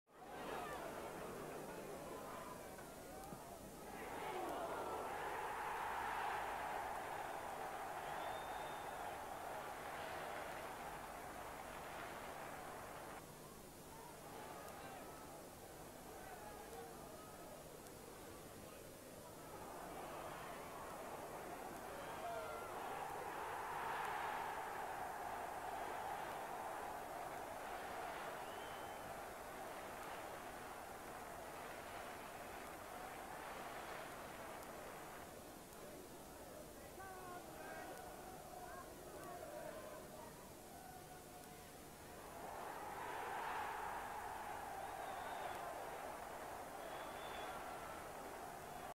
Nottingham Forest 3 Tottenham Hotspur 0, 24-10-1987
Goals from Franz Carr, Neil Webb and Nigel Clough